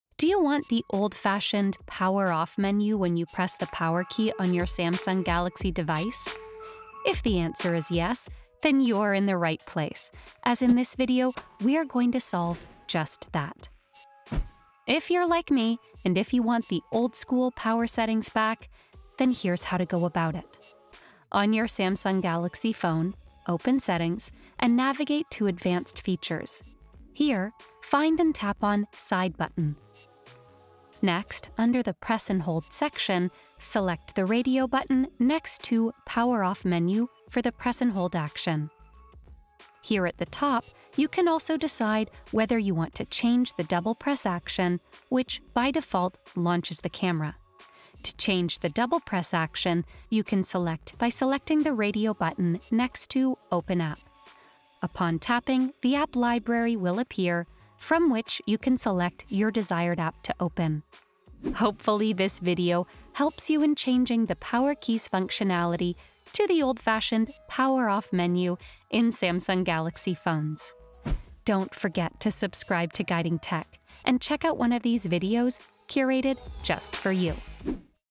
How to Change the Power Key's Function in Samsung Galaxy Phones | Guiding Tech
Normally, pressing and holding the Power Key should take your phone directly to the power menu so you can turn it off or restart it.

But unfortunately, that might not be the case depending on your Samsung Galaxy smartphone you possess.

In this step-by-step tutorial, we’ll walk you through the process of redefining the power key’s function. No need to dive into complex settings—this feature is right at your fingertips.